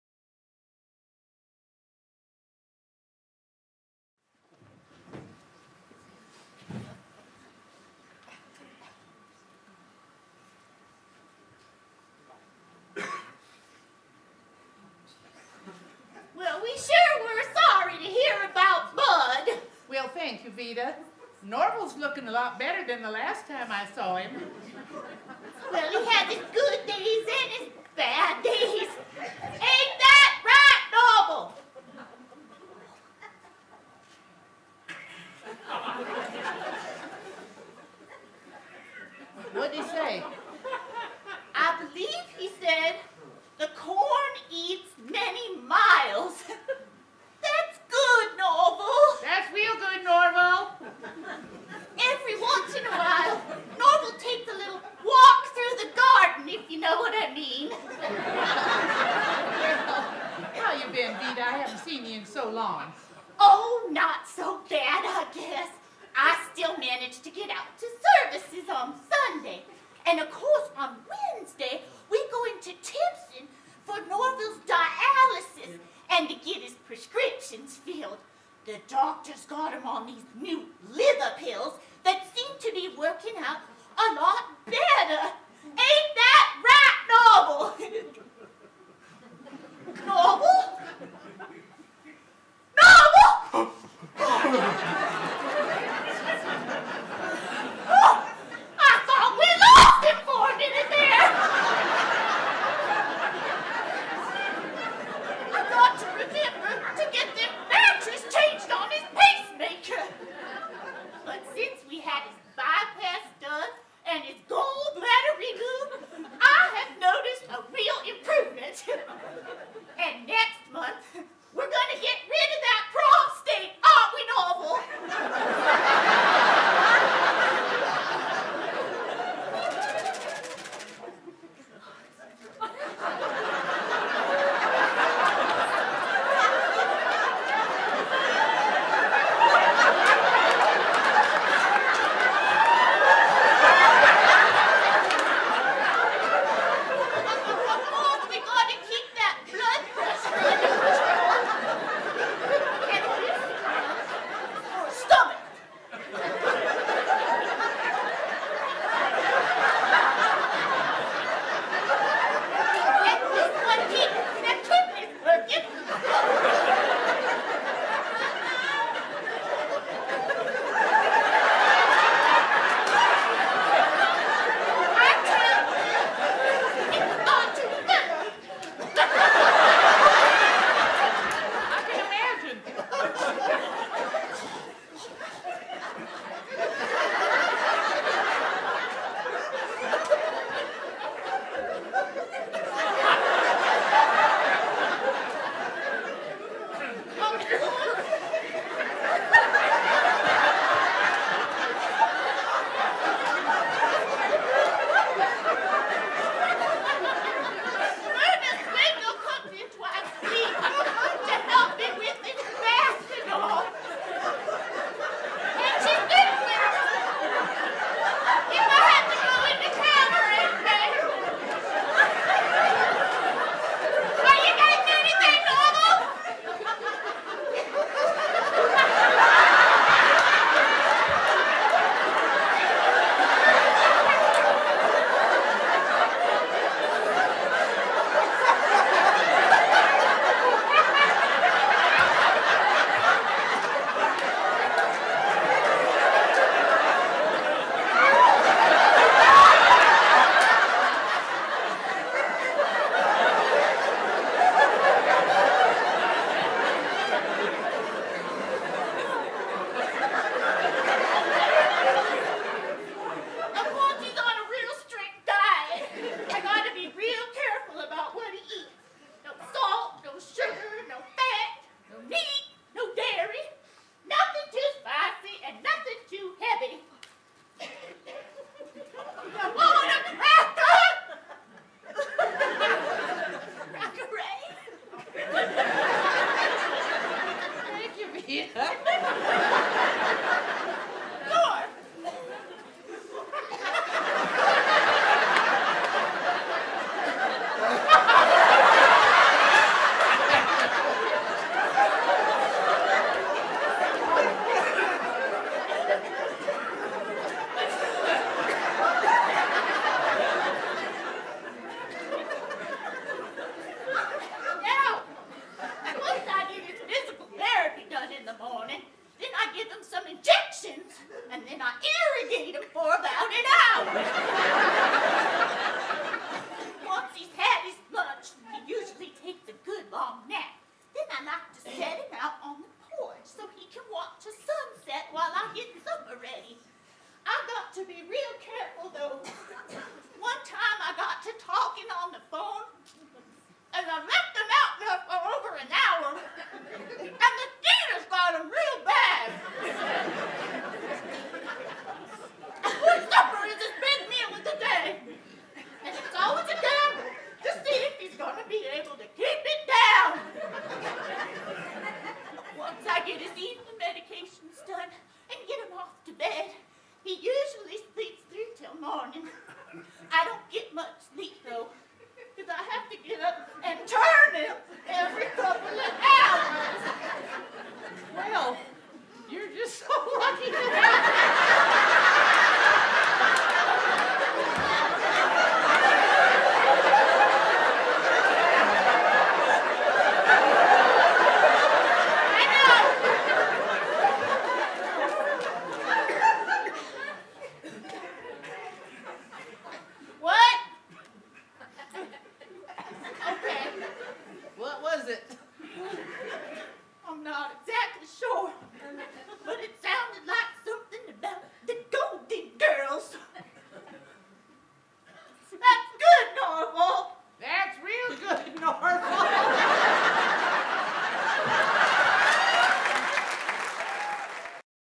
Dearly Departed
A scene from the cast of Dearly Departed at Billings Studio Theatre! Directed by Kelly Martin.